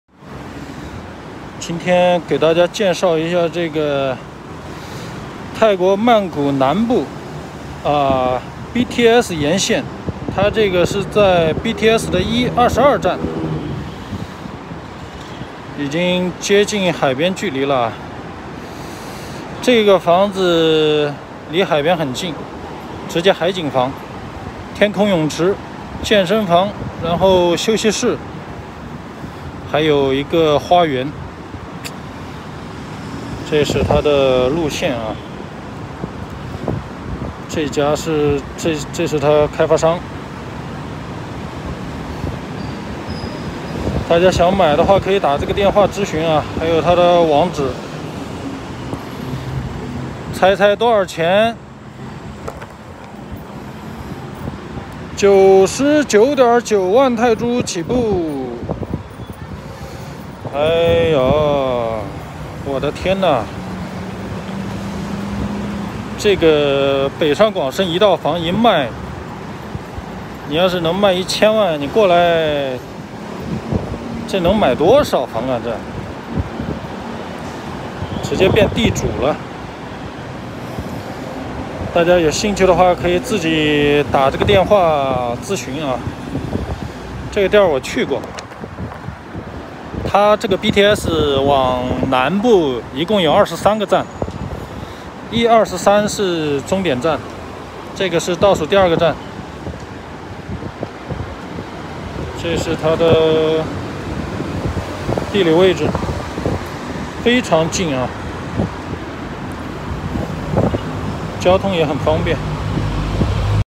曼谷南部海景公寓仅99.9万泰铢合人民币22万元距BTS仅220米
大家可以自己联系，本人不是中介，视频仅记录生活分享给需要的朋友